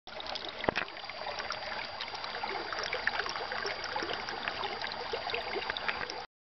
Хозяйка горы
Копнул лопатой, а из под земли зеленая струйка. Пугает хозяйка горы или предупреждает. А может? Может наоборот знак дала. Здесь копать надо.